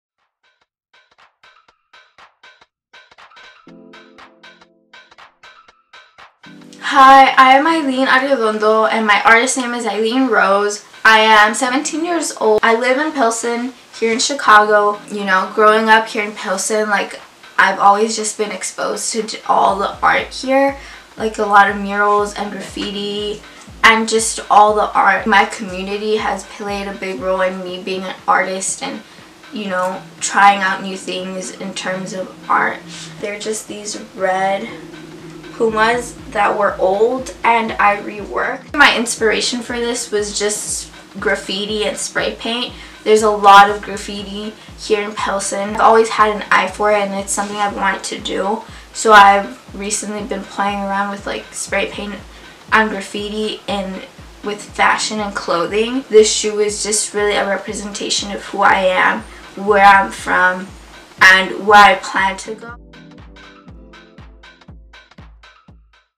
MCA Chicago X Virgil Abloh Design Challenge | Aylin Rose
I entered the MCA Chicago X Virgil Abloh Design Challenge, and made this YouTube version to upload on here. Below is my description of my submission and the old, boring shoes I remade.

I am Aylin Arredondo, but my artist name is Aylin Rose. I am 17 years old, and I am from Pilsen in Chicago. Growing up in Pilsen, there is a lot of art, graffiti, and murals. The art is very beautiful and inspiring, and seeing the graffiti in my neighborhood sparked an interest in me to get creative in my own way using spray paint. I have made a few clothing pieces and have made my backdrops with spray paint and doing this sort of spotted/splash look on my pieces and it has become my signature look, and I have fallen in love with it. I had this old pair of red Puma sneakers that were old, dirty, and ultimately boring. I decided to do my signature spray paint look on the shoes with black, hot pink, and a light pink metallic finish. I also spray painted the shoelaces and used a variety of colors to add dimension to the shoe and make them stand out more. I created my own stencils with A. Rose, which is short for Aylin Rose, and I also created a stencil of a rose. Roses are very significant in my life because of the quote “De la espina y el dolor, crece la flor,” which means “from the thorns and pain, the rose grows.” This is a significant quote because it shows that the pain and struggles in life make us all stronger and that we will overcome any difficulties. It shows how we must struggle and work hard, to then reach our highest potential. I enjoyed this prompt because I was able to work with what I have and made me realize that as an artist, I am capable of turning anything into art. This has taught that you don’t have to always go out and buy new material, but it is about how you innovate and create your artwork. This is an important concept to grasp as an artist, entrepreneur, etc. because a lot of people wait for the perfect moment to start, but the truth is that there is never a perfect moment, and we have to work with what have at the moment to make things happen for ourselves.